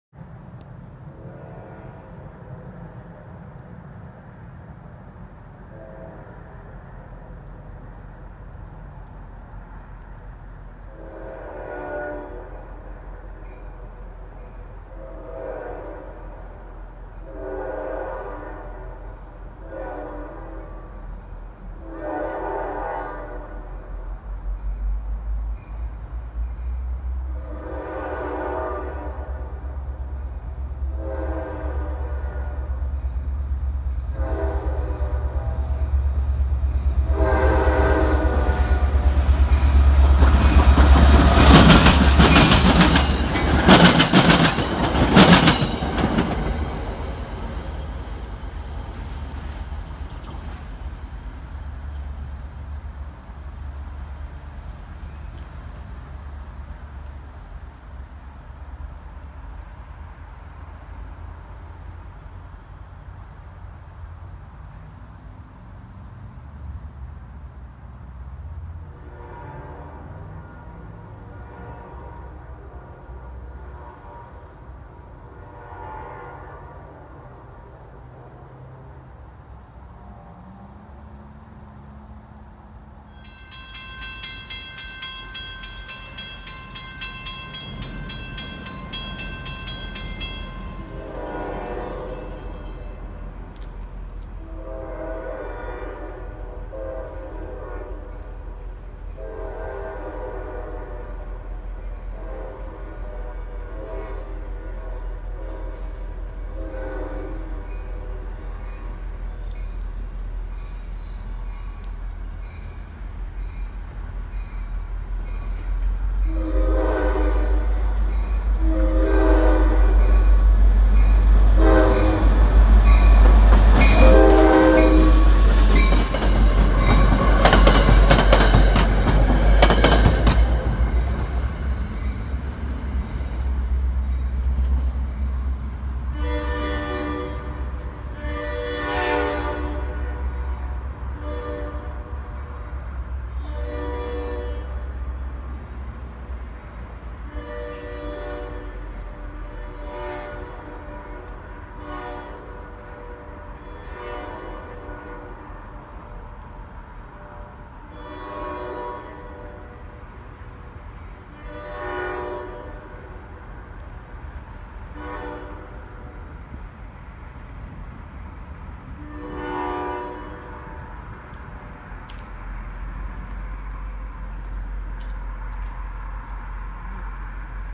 Full HD 60 FPS: Chasing MM-08 from Red Bank to Belmar 2/4/19
In this video - GP40PH-2B #4200 leads MM-08 through Red Bank & Belmar. I chased them to Bay Head - despite getting caught in a massive detour on Rt 71 & missing them pull into Bay Head station yard by a couple seconds due to a traffic accident but I was able to film part of the yard drill before heading home. Drill will be seen in the next video.